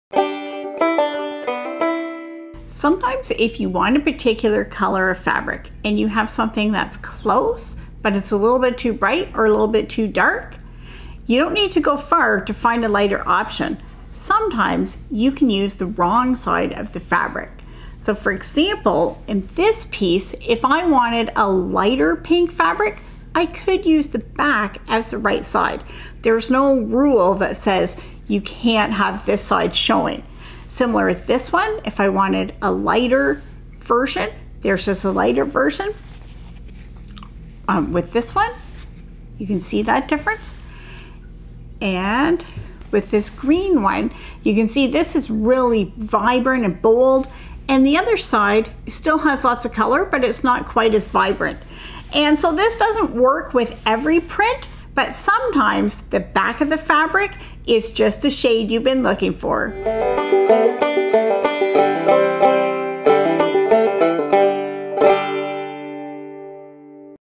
How can I find the perfect shade of fabric?
If you have a fabric that is close to the colour that you want, sometimes using the "wrong side" of the fabric will get you the shade you are looking for.